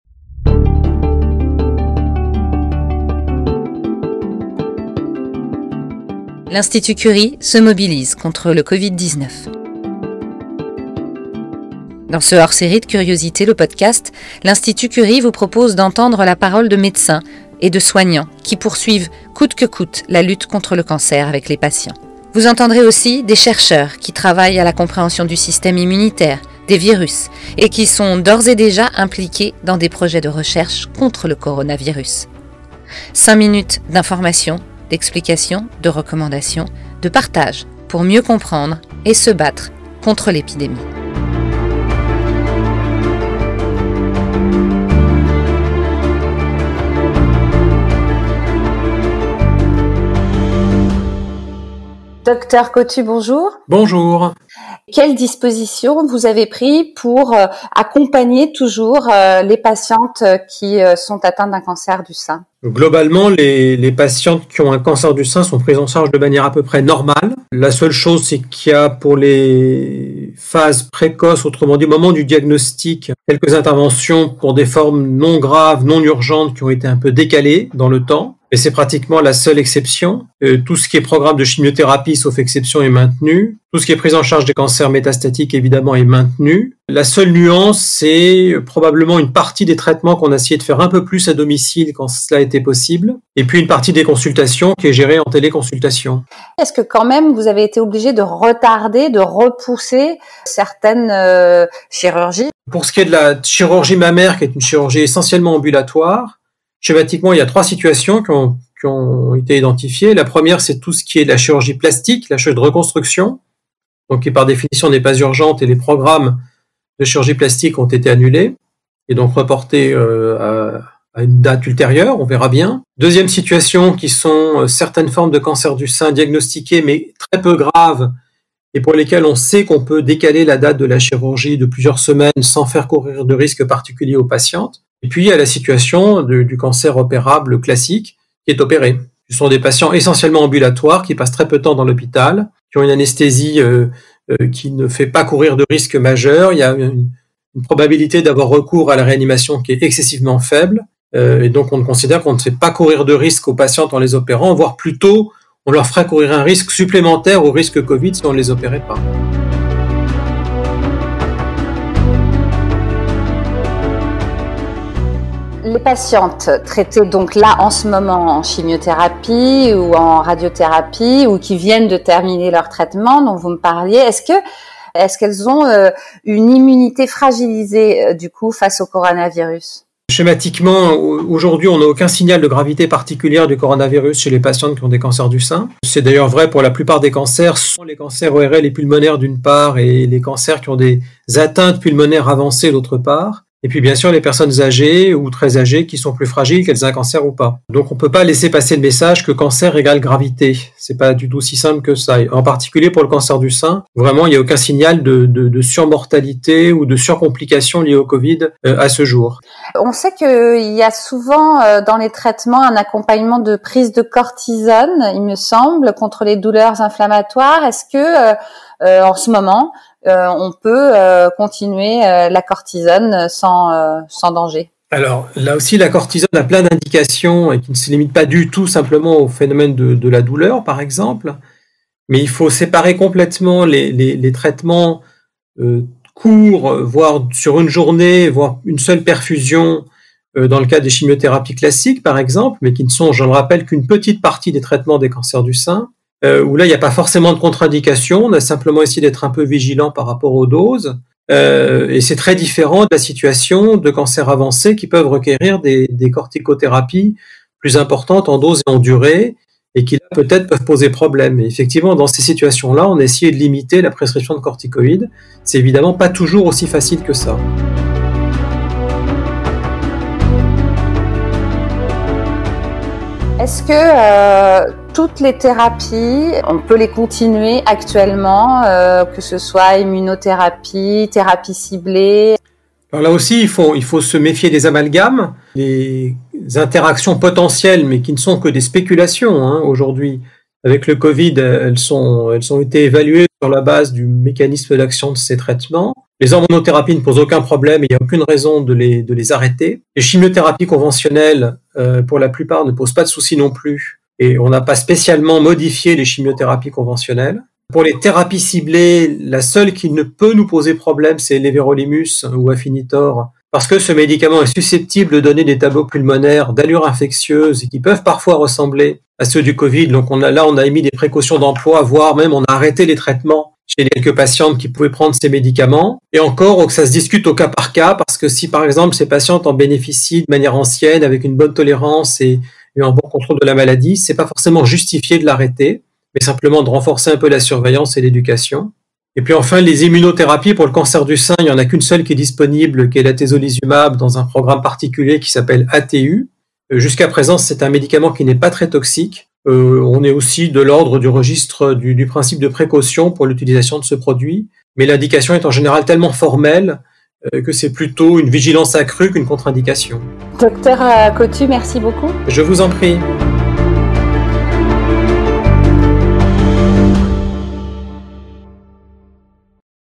Covid-19 et cancer du sein : entretien avec le Dr Paul Cottu.
Le Dr Paul Cottu, chef adjoint de notre département d’oncologie médicale, prend la parole sur notre podcast et revient sur l'organisation mise en place pour la prise en charge des patientes atteintes d'un cancer du sein durant l'épidémie de COVID-19.